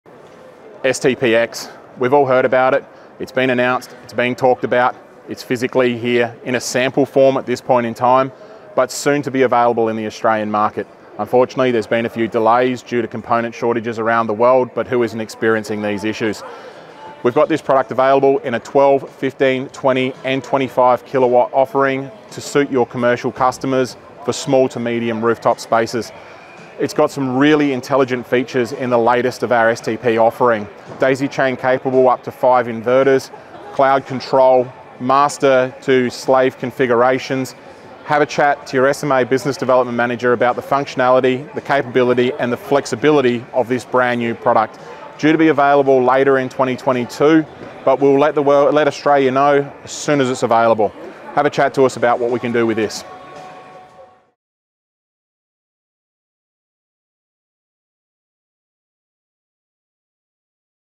Energy Next 2022: Overview Sunny Tripower X
Future solutions, pre-installed. The Sunny Tripower X is the latest in commercial energy use and the new centerpiece of the tried-and-true SMA Energy System Business. With the integrated System Manager, the Sunny Tripower X can not only monitor the entire system, but also control and regulate it independently.